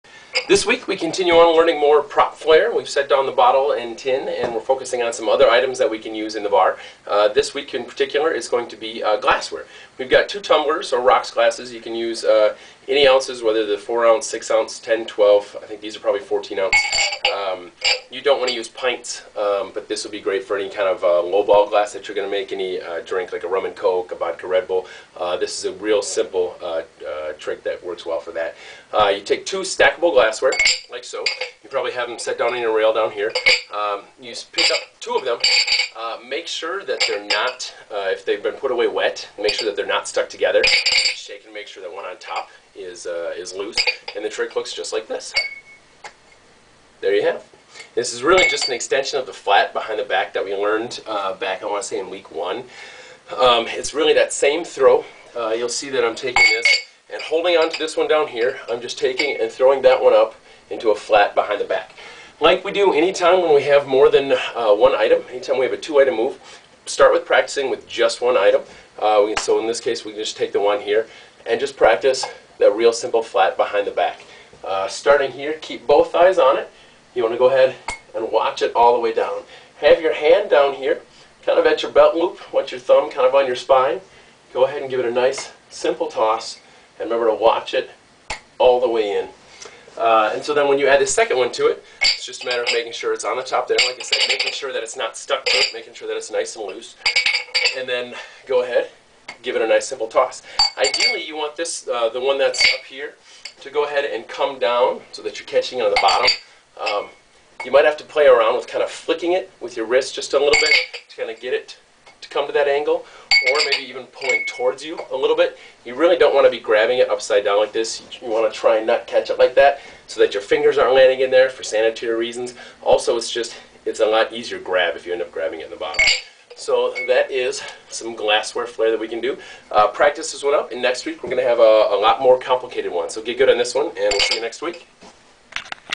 Flair Bartending Lesson 25: Glassware #1
Stop watching the old version of this move and watch the new flair bartending lessons

Practice this at home!! Be sure to practice this enough until you're comfortable with it before you attempt it at work. Breaking glassware at work is generally a bad idea.

This week's move can be a really good gauge to test your customers' response to flair.  I use this simple trick at least 50 times a night. If I get no response or a negative response from my guests, I simply go into making the drinks quickly and tastily. If I hear "Hey, that was cool" or "Did you see that?" or anything else positive from my guest, I know I can perform a little more exciting flair for them. This move is a great way to get a "read" on your patrons.
Cheers!
Chris